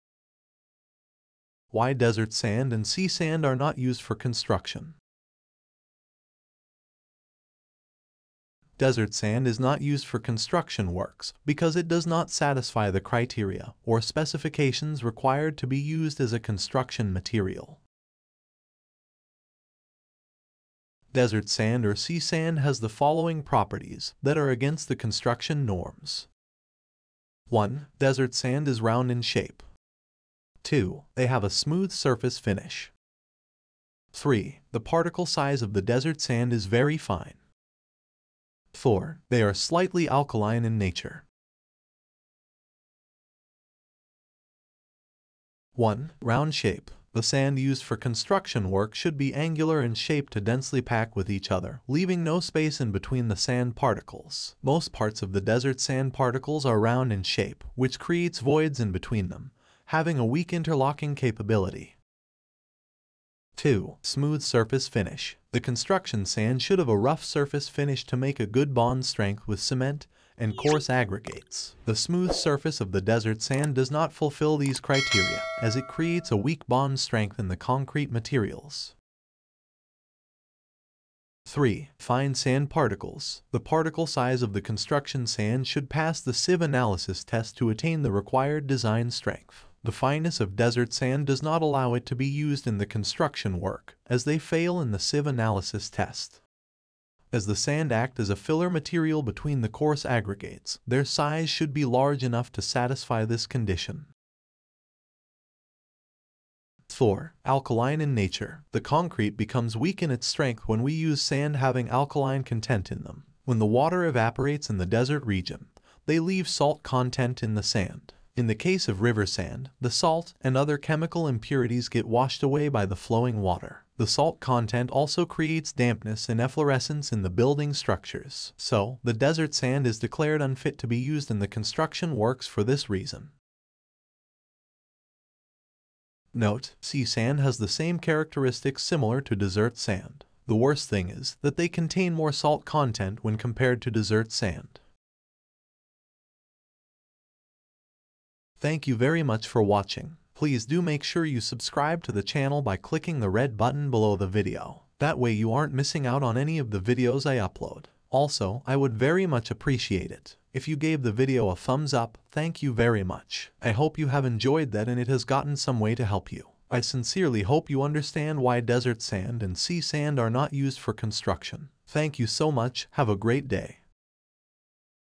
Why Desert Sand & Sea Sand Are Not Used For Construction? | Civil Engineering Basic Knowledge
In this video, we'll be discussing why desert sand and sea sand are not used for construction. We'll be discussing the different properties of these sands and how they affect construction.
If you're looking to learn more about construction, then this video is for you! By the end of this video, you'll know why desert sand and sea sand are not used for construction and what the different properties of these sands are.